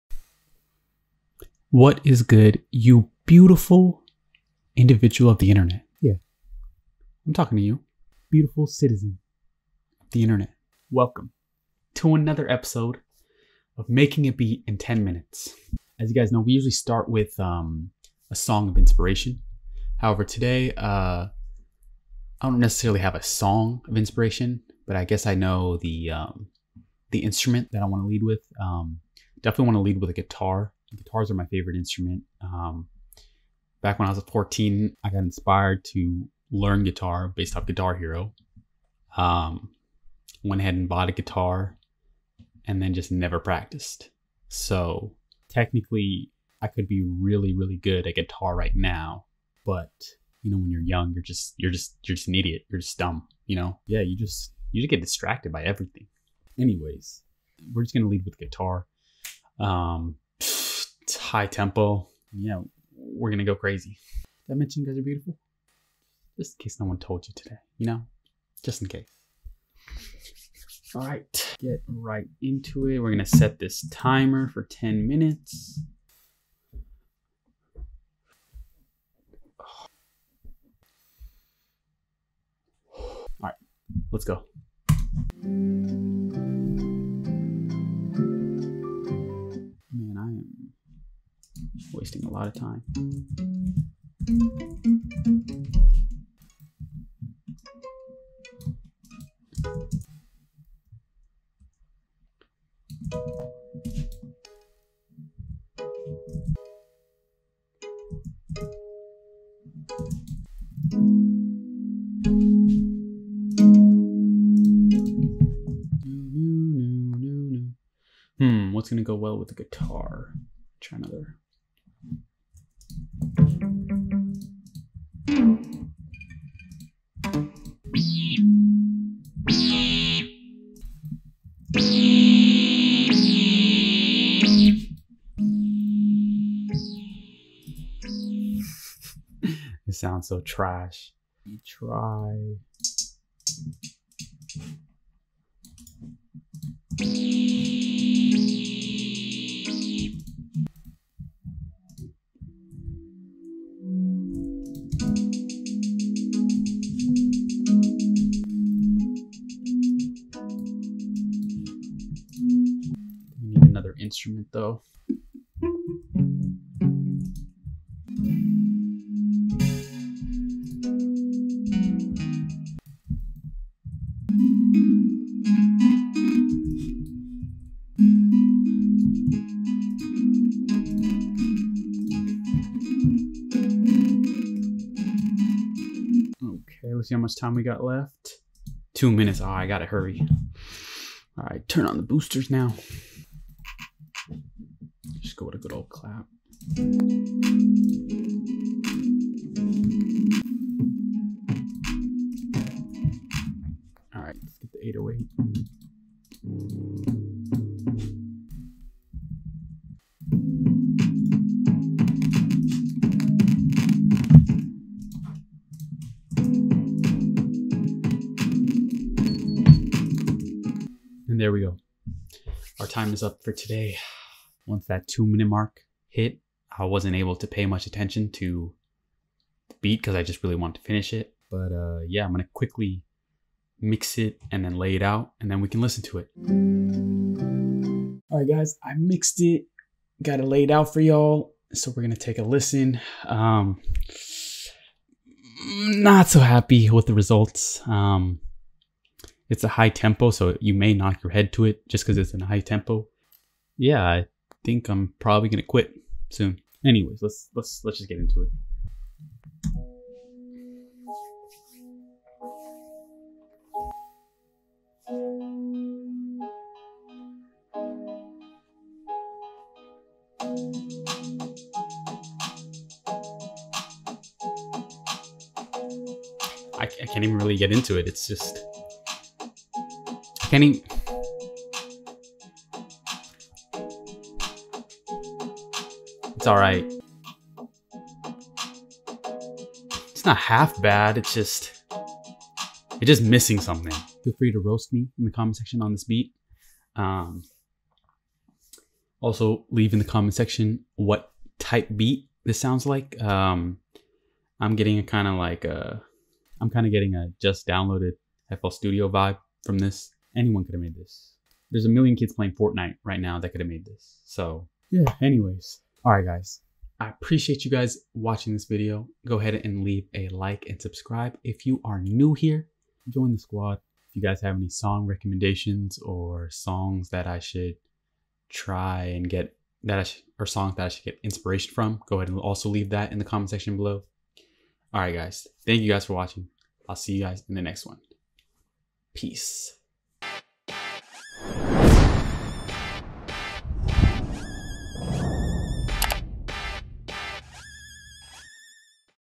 Making a beat in 10 minutes — 3
I tried to make a fire beat in 10 minutes.

This is a series I'm going to continue making, so leave recommendations of songs I should check out or get inspiration from.

Your suggestion could be in the next video!